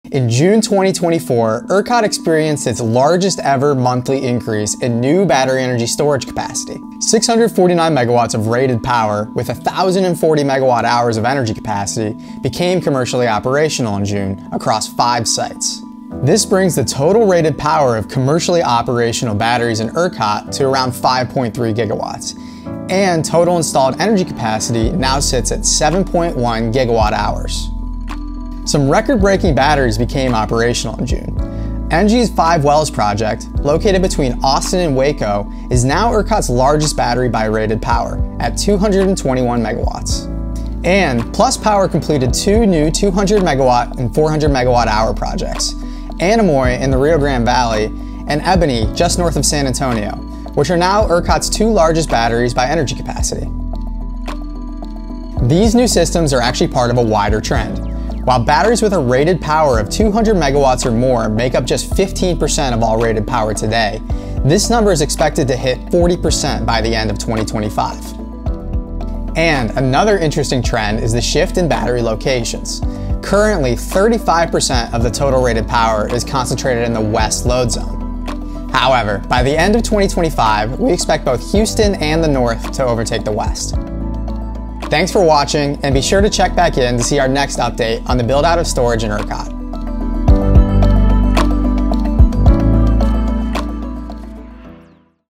ERCOT battery buildout: Record-breaking new energy storage (June 2024)
In June 2024, ERCOT experienced its largest-ever monthly increase in new battery energy storage capacity.

649 MW of rated power - with 1,040 MWh of energy capacity - became commercially operational across five sites.

This brings the total rated power of commercially operational BESS to 5.3 GW. Since the beginning of 2023, this figure has grown by 2.7x.

June wasn’t only the largest-ever increase in rated power and energy capacity in ERCOT.

It also saw the commissioning of the largest-ever battery projects - both in terms of rated power and energy capacity.

ENGIE’s Five Wells project became the largest battery by rated power in ERCOT at 221 MW. This surpassed ENGIE’s 206 MW Libra BESS, which came online in April of this year.

Additionally, Plus Power completed two projects that now share the record for the largest energy capacity of any battery in ERCOT: Ebony, located just north of San Antonio, and Anemoi, near McAllen in the Rio Grande Valley.

Both are ~2-hour systems with 400 MWh energy capacities.

Elsewhere, Hunt Energy Network continued to add to its network of one-hour, 9.9 MW resources.

In June, Farmersville West 1 and Mainland became commercially operational. This brings Hunt’s total number of battery energy storage systems in commercial operations up to 24.

As total rated power grew to 5.3 GW in June, total energy capacity hit 7.1 GWh.

This brings the average duration of battery energy storage systems in ERCOT to 1.34 hours.

And batteries are getting bigger in general.

In April 2024, the first 200+ MW battery in ERCOT reached commercial operations. In June, three more new batteries (mentioned above) crossed that same threshold.

The average size of battery energy storage systems in ERCOT now stands at 42 MW. This represents a 31% increase from the 32 MW average from exactly twelve months ago.

Battery energy storage systems larger than 200+ MW currently account for 15% of total rated power today.

But, by the end of 2025, we expect this proportion to rise to 40%.

BatteryEnergyStorage hashtag#ERCOT hashtag#RenewableEnergy